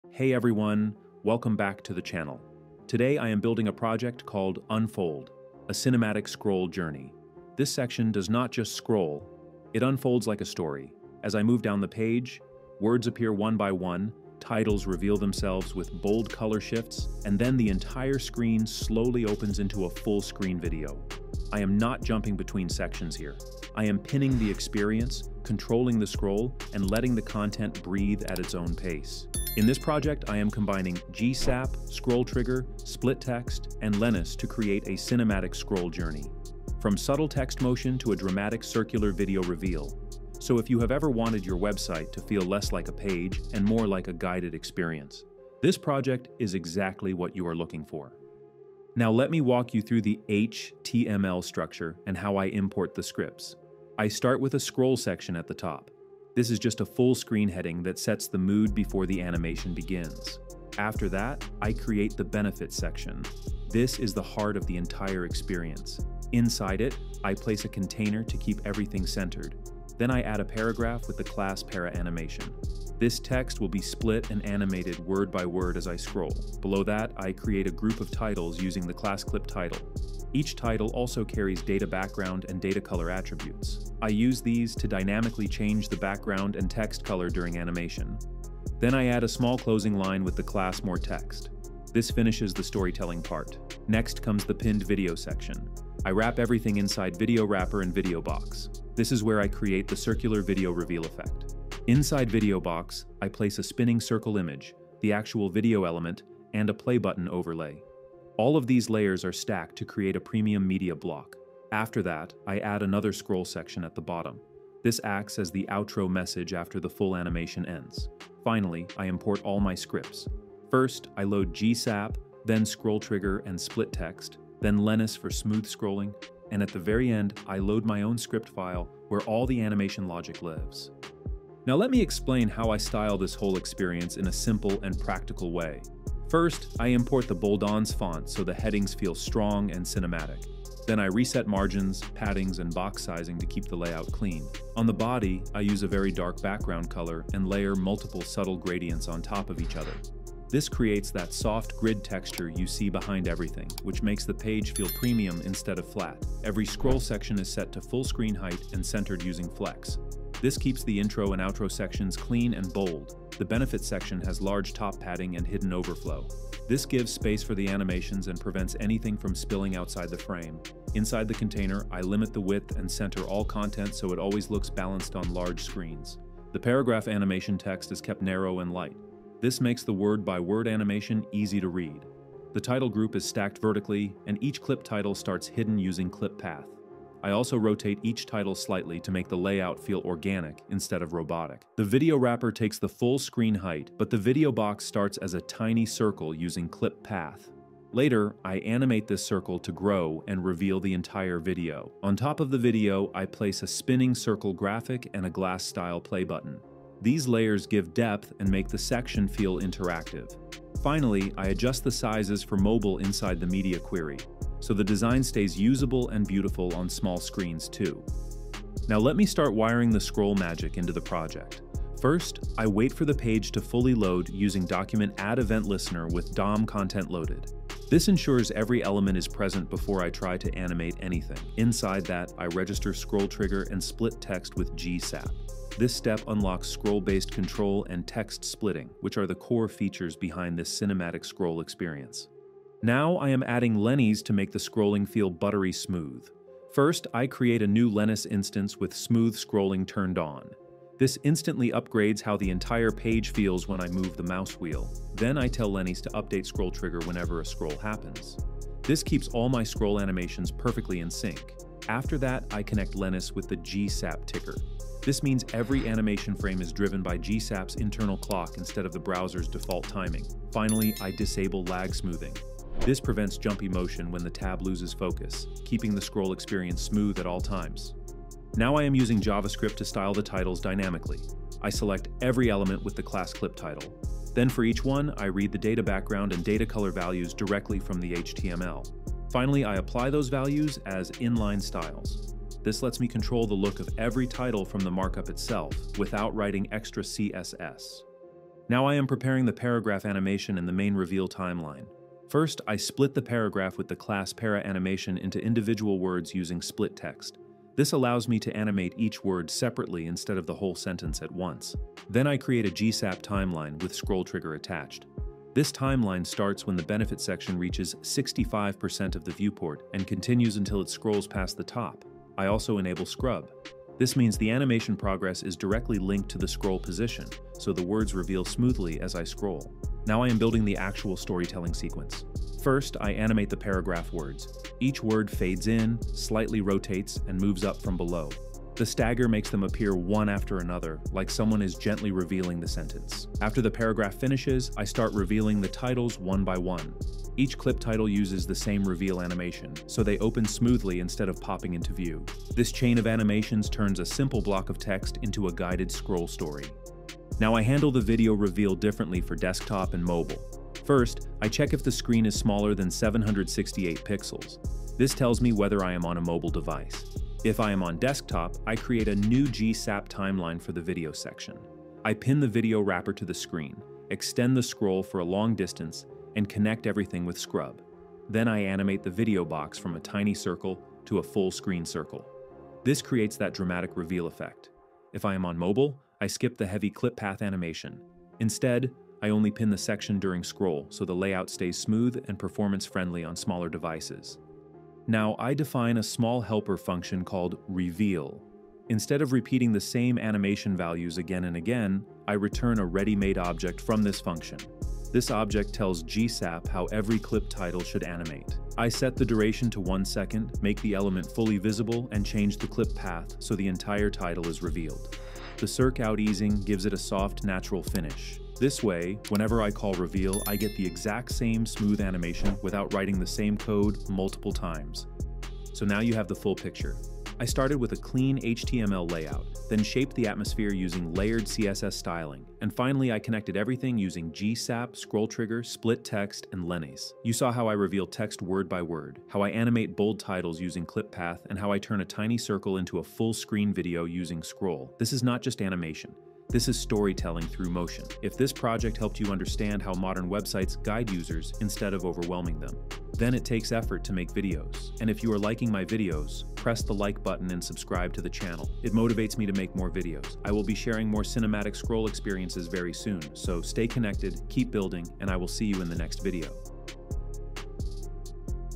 Build Cinematic Scroll Stories That Stop People Scrolling
In this video, we move beyond basic animations to create Motion-Led Storytelling controlled by scroll. Learn how to build a Cinematic Scroll Storytelling experience that transforms a standard webpage into an Immersive Scroll Reveal.

We dive deep into advanced GSAP ScrollTrigger techniques to master these modern UI patterns:

✅ Pinned Story Sections: How to lock content while the narrative unfolds.
✅ Clip-Path Video Unmasking: Create stunning reveals that react to user interaction.
✅ Sequential Content Unfolding: The secret to pacing your scroll-driven narrative perfectly.

If you want to master Scroll-Driven Narrative Experiences, this tutorial covers the exact code and logic you need. Whether you are a creative developer or a UI designer, this GSAP scroll animation guide will level up your frontend skills.